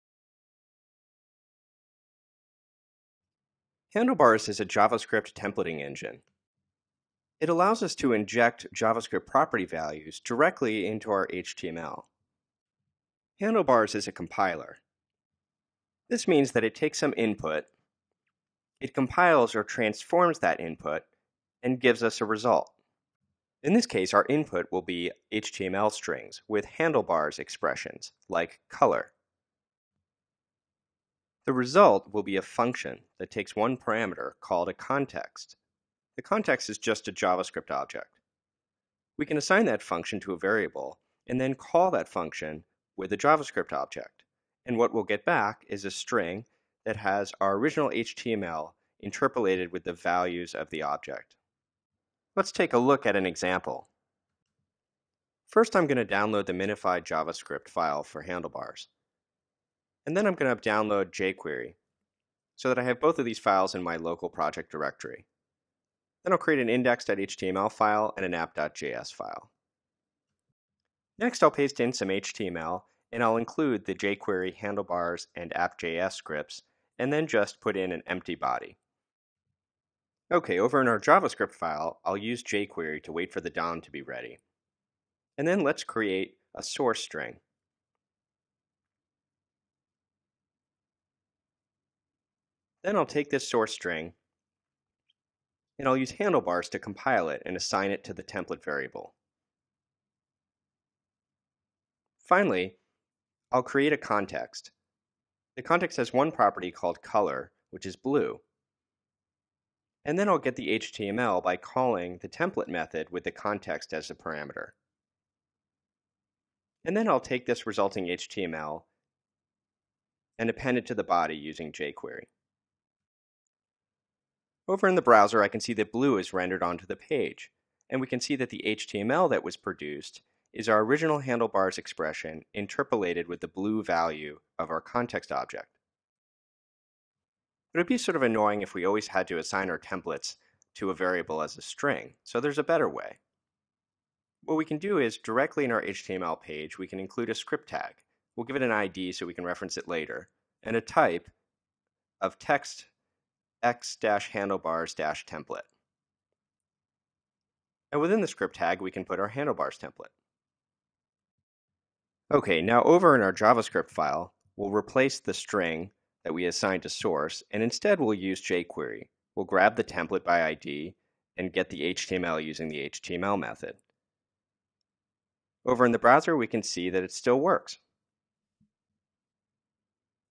Handlebars - How it works
Handlebars is a popular JavaScript template engine used in Meteor, Ember and other frameworks. It allows us to inject results from a function or object property directly into our html. In this episode we'll look at how Handlebars works using templates and a compilation process.